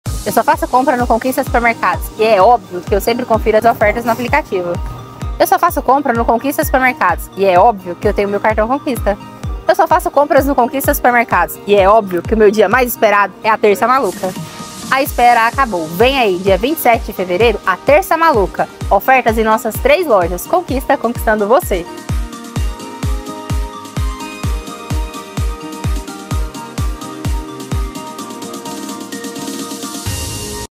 AGUARDE é OBVIO que é a TERÇA MALUCA!
A TERÇA MALUCA Conquista Supermercados está chegando! Se prepare porque só aqui tem oferta de arrasar e preços baixos de verdade! SOMENTE NESTA TERÇA-FEIRA DIA 27/02/2024! AGUARDE!